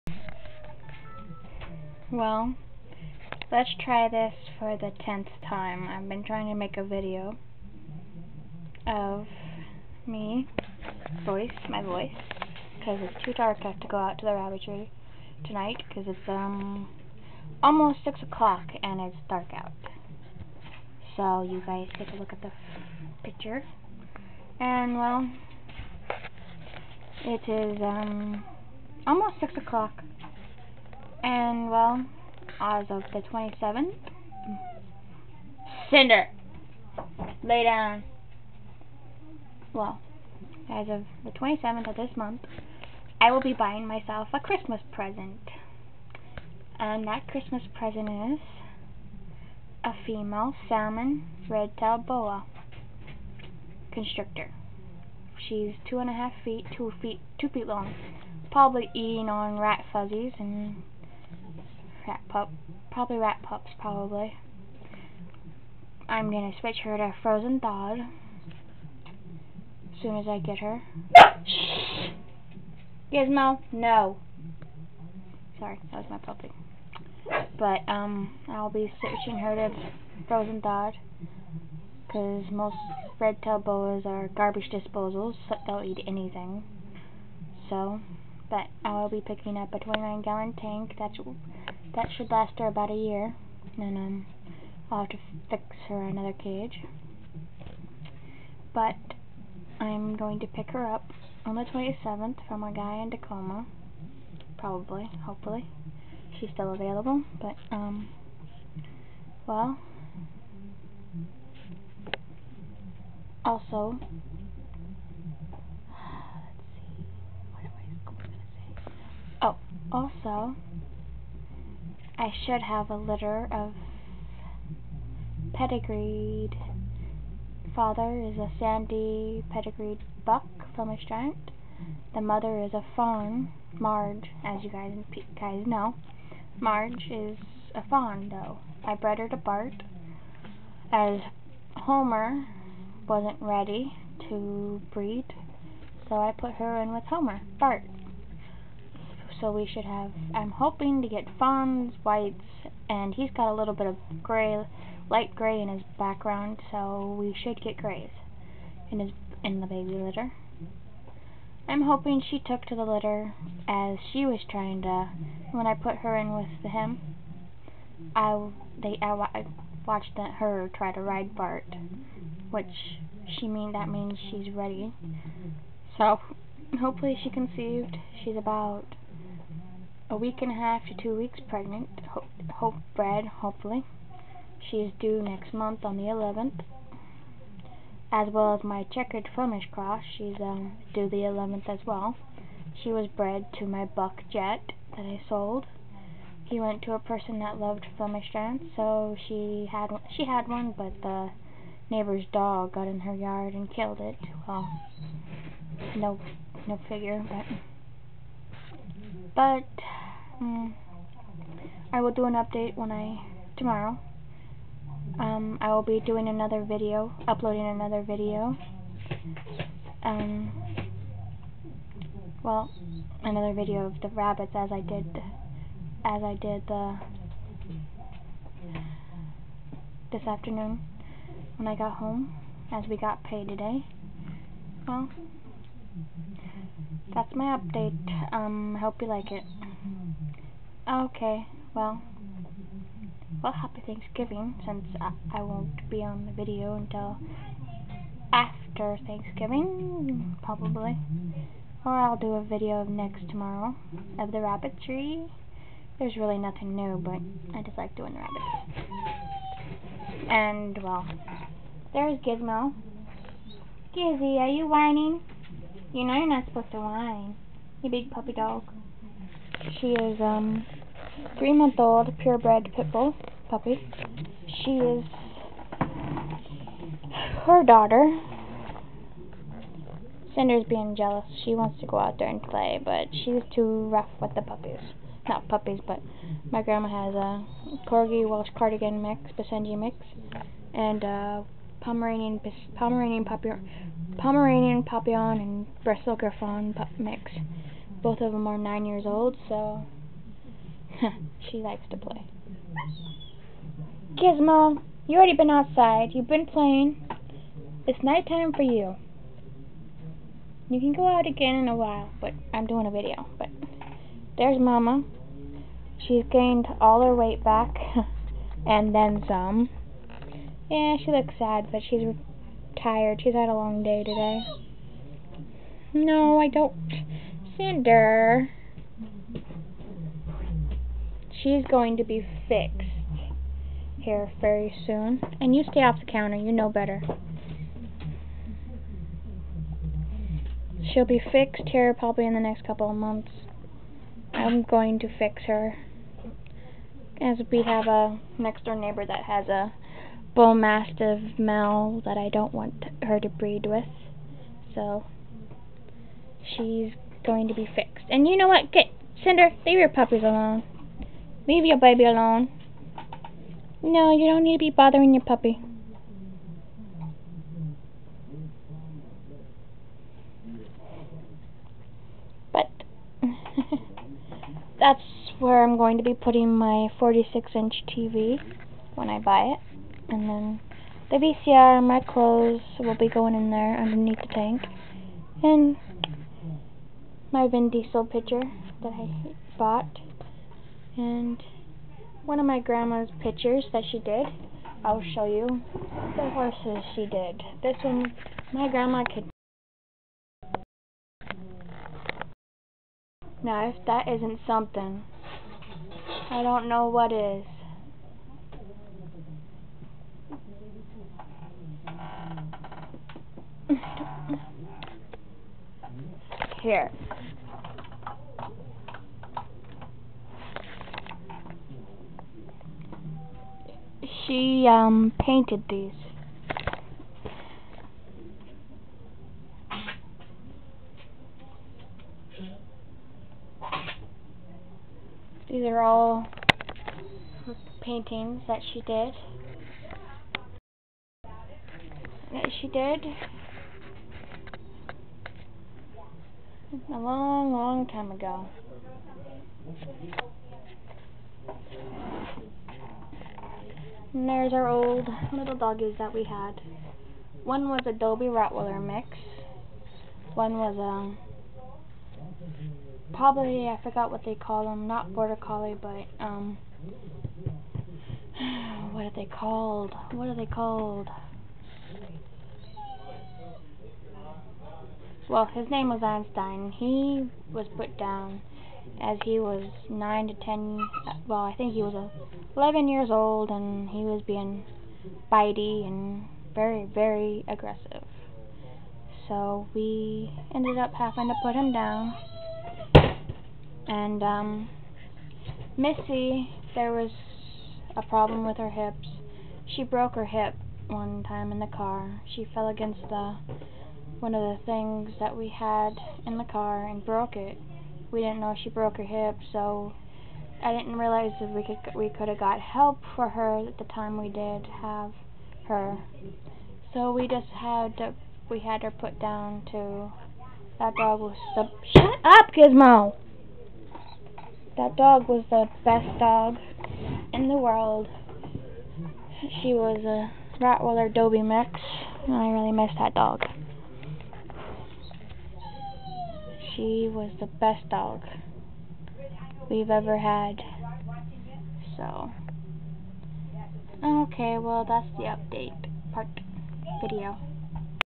A little confersation about what is happening on the 27th of november of this month
Hi this is a little update on what is going on in my life. I will be getting a female red tail boa constrictor she is 2ft long she is a salmon red tail. she has peach coloring all around her.

Most of you know me that i raise pure bred flemish giants and one checkered/flemish giant cross well these are safe they are not going to be fed to my snake so please no hate comments please.

I have always wanted another boa for a long time I had red tails when i was  17 years old my first snake was a baby pastel red tail boa. but now i am in a place where I can get another red tail boa. so i will be picking up a female baby boa here in 6 more days and down the road i will find her a mate when we find are new home that we are going to permantelly live in. lol. :) :)